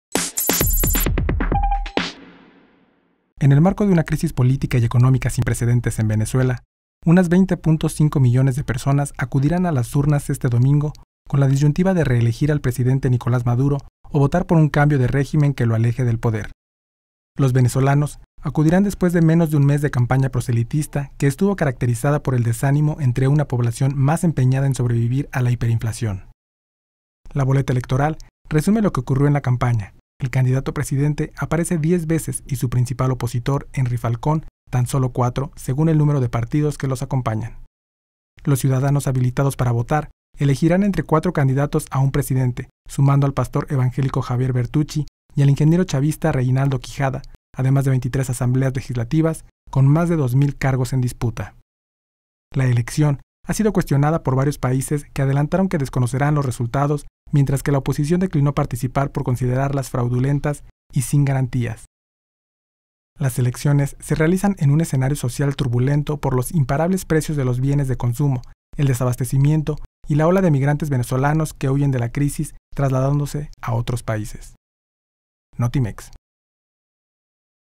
Venezolanos votarán en medio crisis política y económica sin precedentes
Los venezolanos acudirán a las urnas después de menos de un mes de campaña proselitista, que estuvo caracterizada por el desánimo entre una población más empeñada en sobrevivir a la hiperinflación